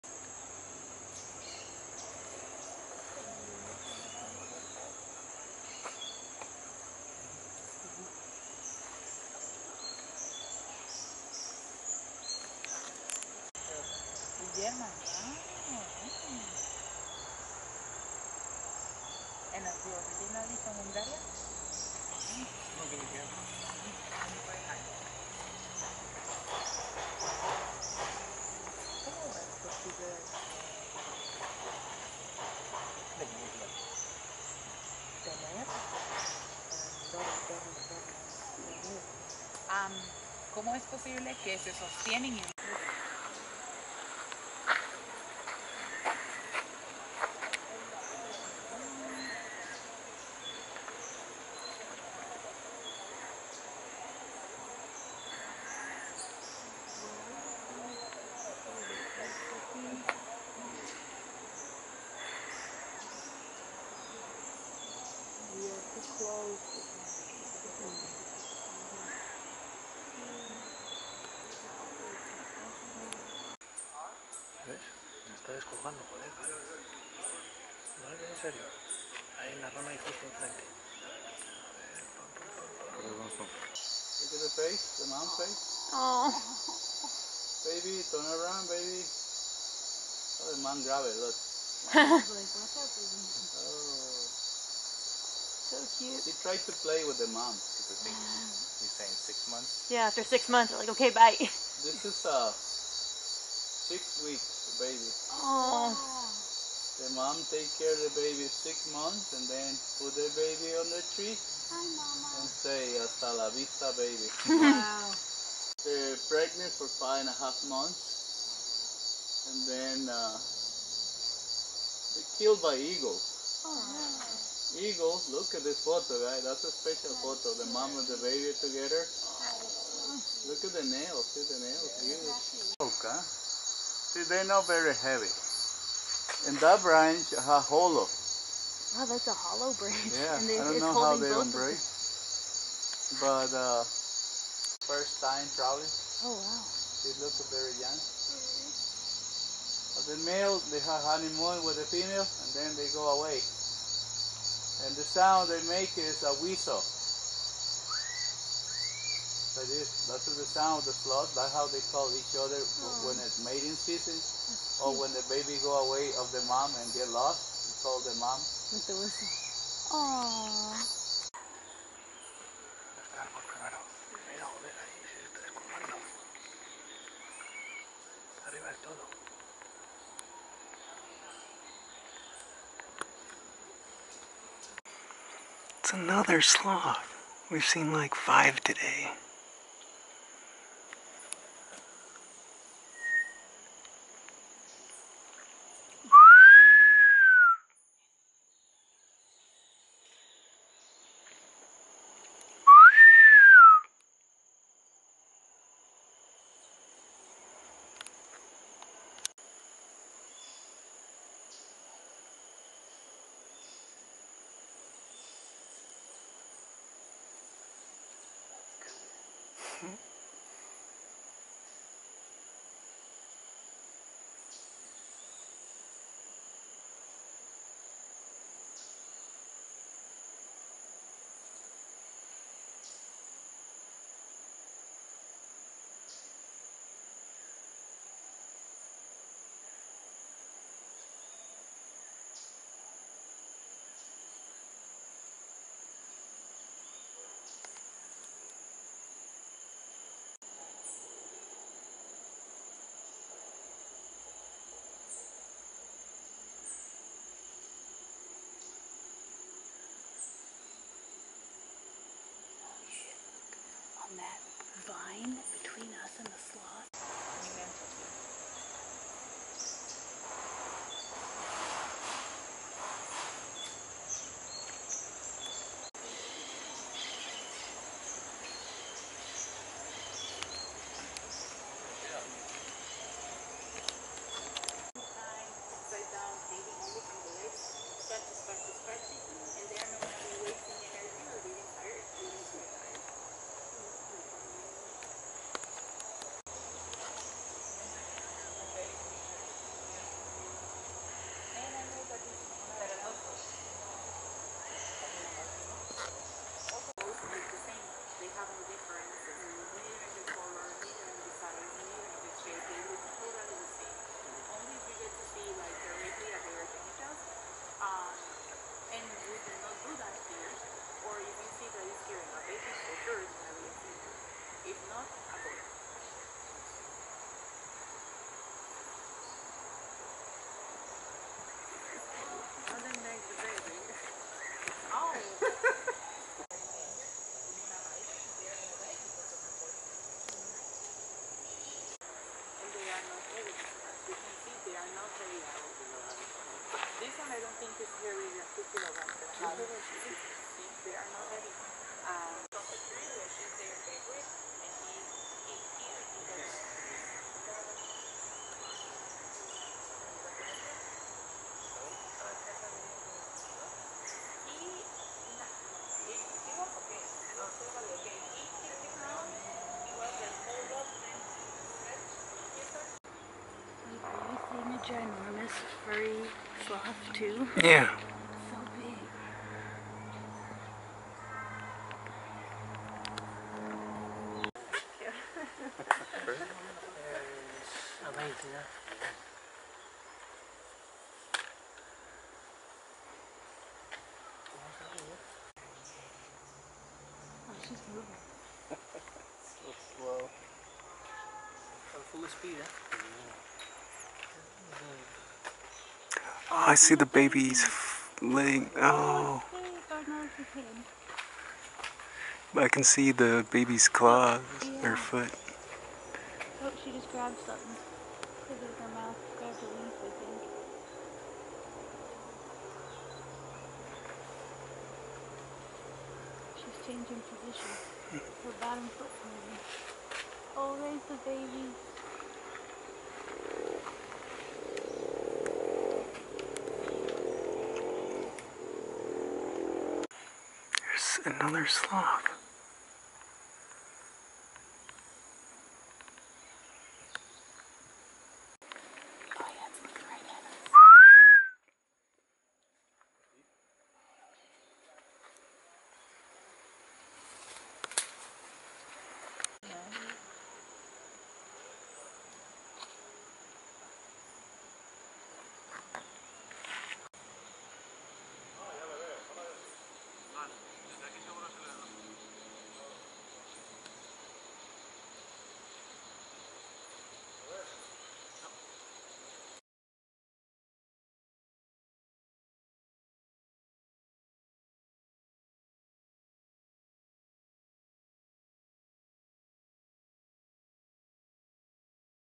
The amazing sloths of COSTA RICA!
Seeing wild sloths has been a bucket list item for us, and they did not disappoint! They are so interesting and are very cute. Give us a 'Like' if you love sloths!